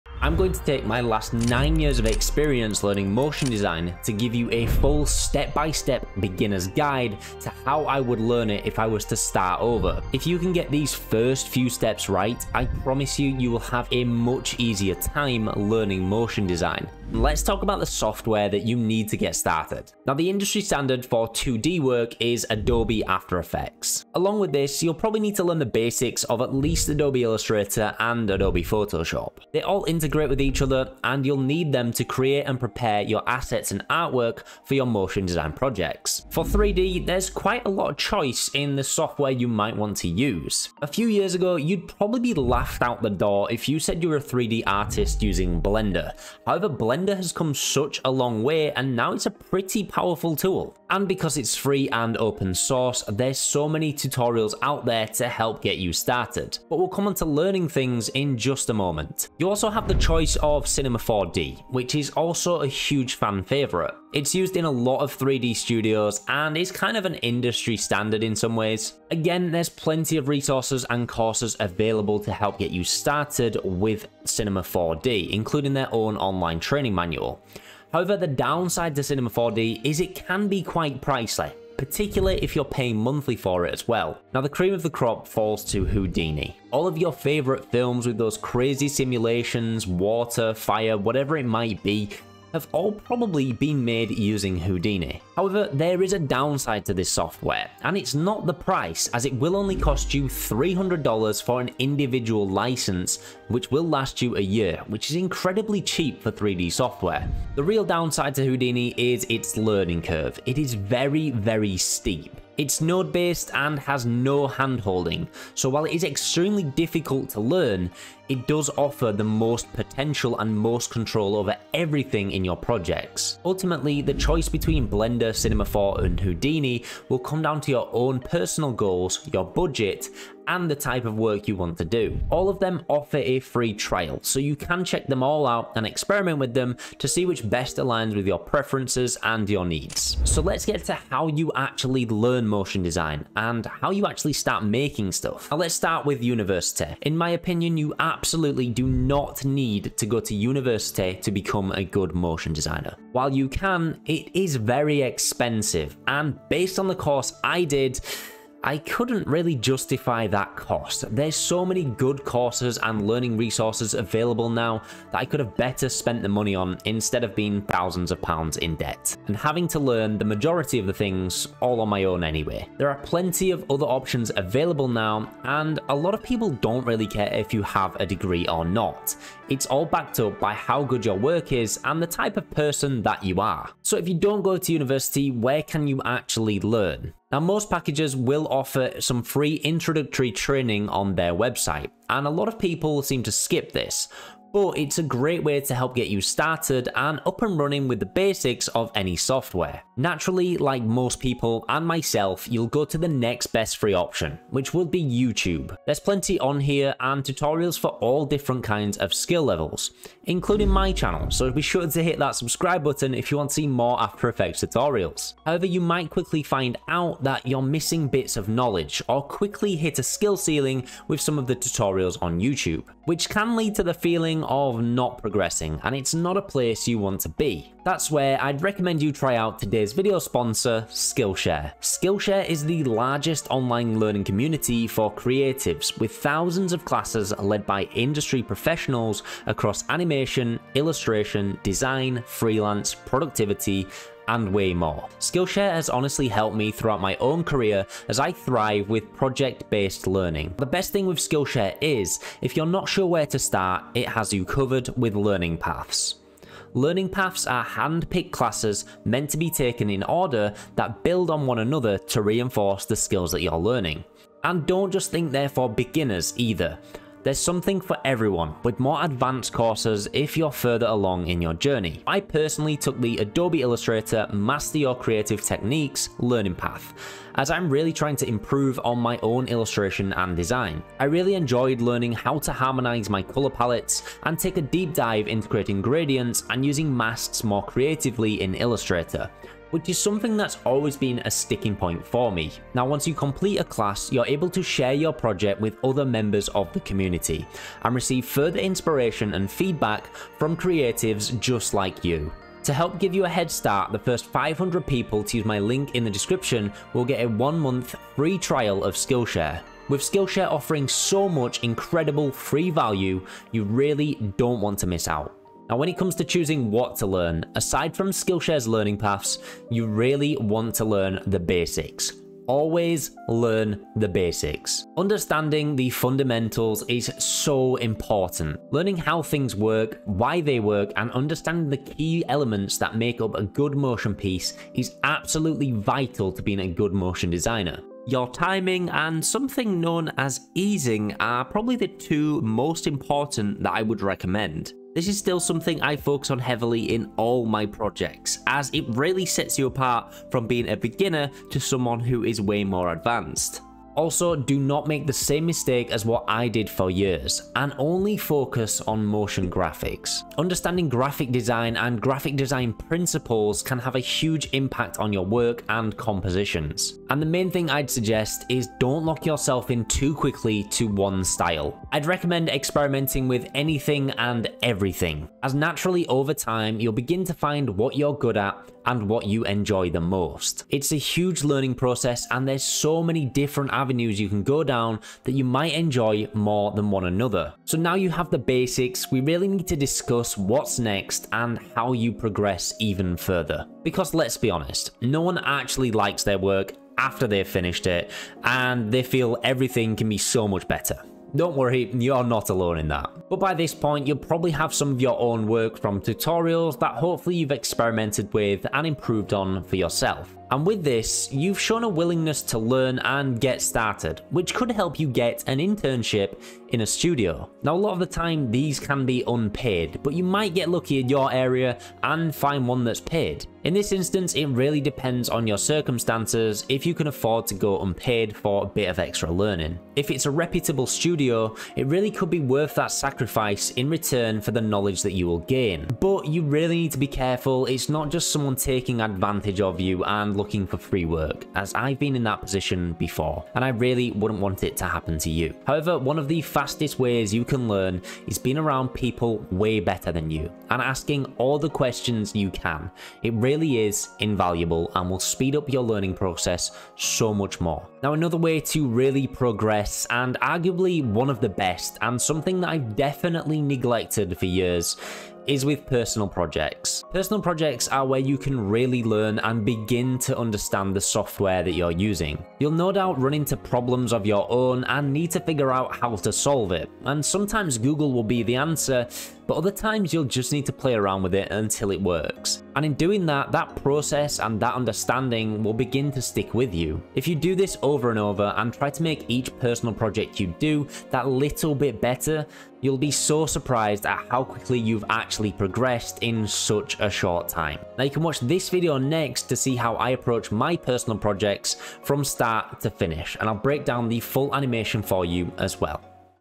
HOW To Learn Motion Design In 2026 From ZERO
📃 TABLE OF CONTENTS:
0:00 - Intro
0:17 - Software
0:36 - 3D Software
2:20 - How To Learn
5:15 - What To Learn
5:54 - DON'T DO THIS
6:27 - How To Progress
7:38 - Don't neglect this